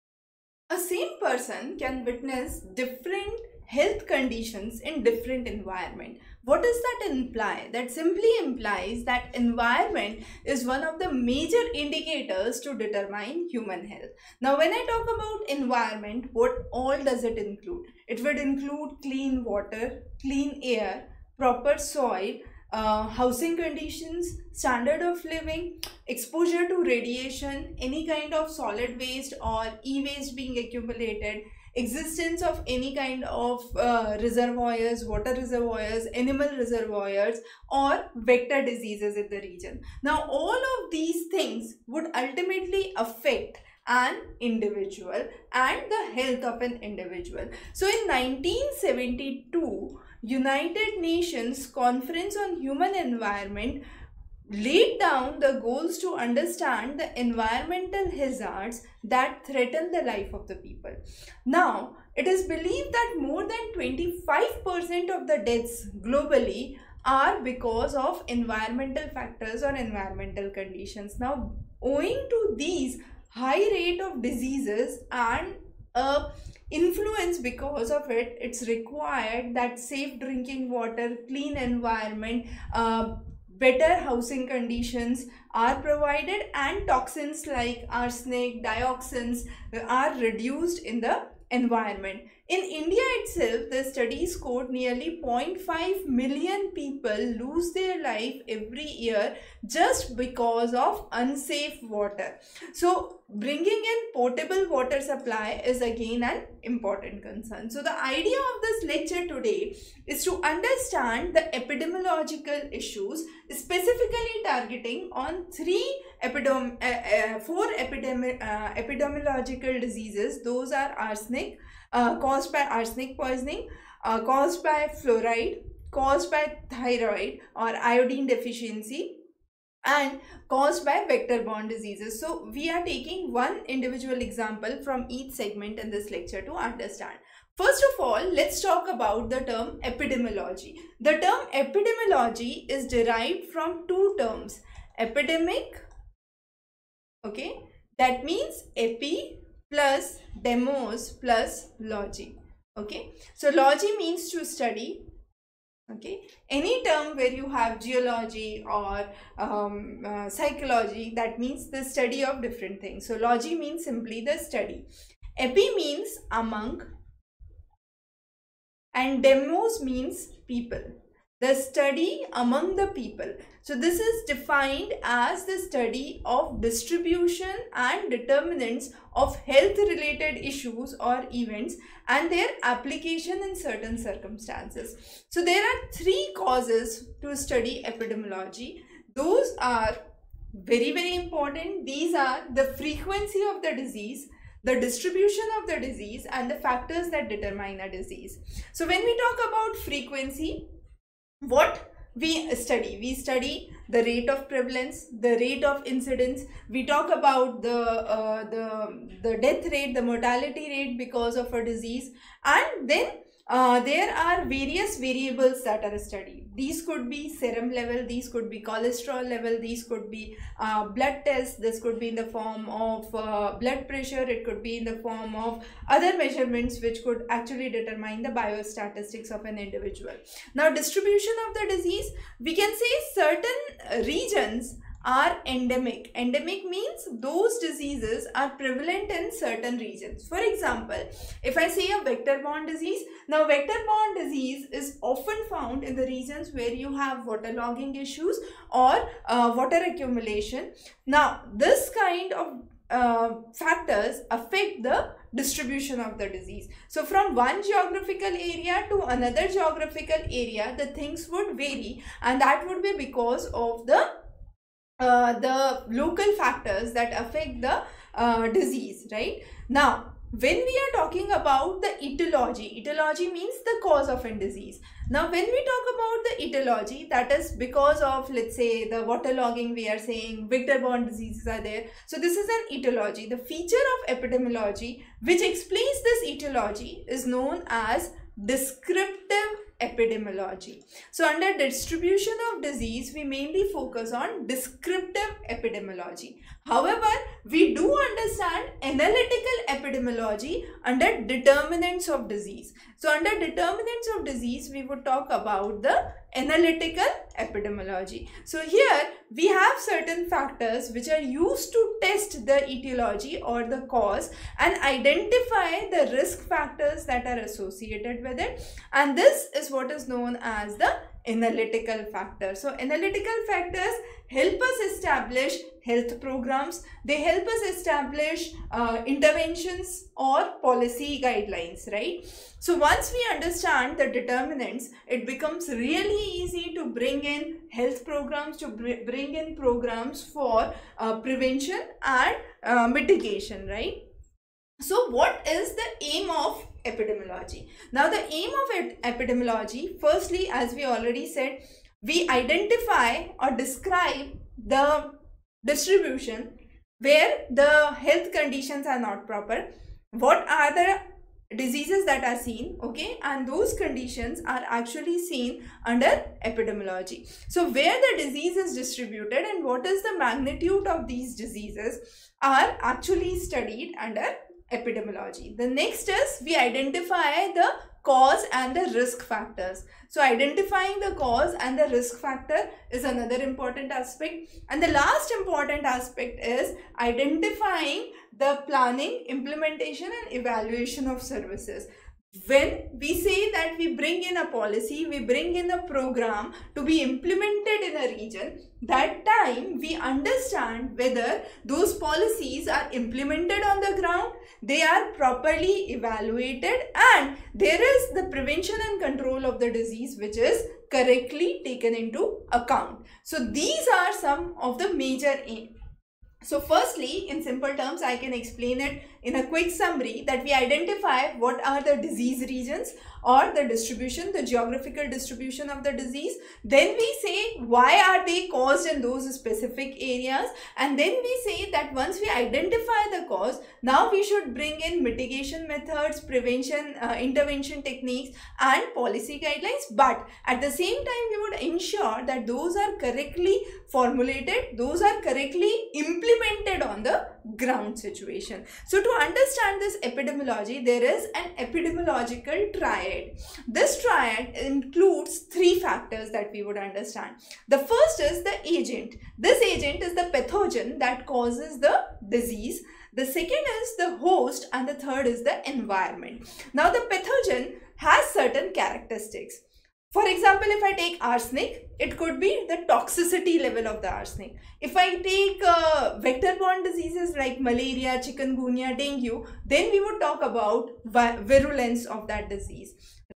Epidemiological Issues: Fluorosis, Arsenocosis, Goitre, Dengue @ doorsteptutor.com UGC Env Science
Epidemiology is derived from the word epidemic, in which epi means ‘among’ demos means ‘people’ and logos means ‘study’.According to John M. Last, epidemiology is defined as “the study of the distribution and determinants of health-related states or events in specified populations, and the application of this study to the control of health problems”. Epidemiological issues are based on three factors.1. Disease frequency 2.Distribution of diseases 3.Determinants of disease
Frequency: form of rates and ratios like, prevalence rate, incidence rate, death rate, etc. health needs, demands, activities, tasks, health care utilizationand variables like blood pressure, serum cholesterol, height, weight, etc and the measurement of disease and health-related events analysed in the domain of biostatistics
Distribution: population by time, place and person. The epidemiologist investigates whether there has been an increase or decrease of disease over time span or whether there is a higher concentration of disease in one geographic area This assessment leads to control or prevent the disease by formulation of aetiological hypothesis. This feature of epidemiology is known as “descriptive epidemiology”.
Determinants of disease:The main characteristic feature of epidemiology is to test aetiological hypotheses and identify the risk factors of disease. This requires the use of epidemiological principles and methods. This feature of epidemiology is known as “analytical epidemiology”. Analytical approachesfacilitate in developing scientifically established health programmes, interventions and policies.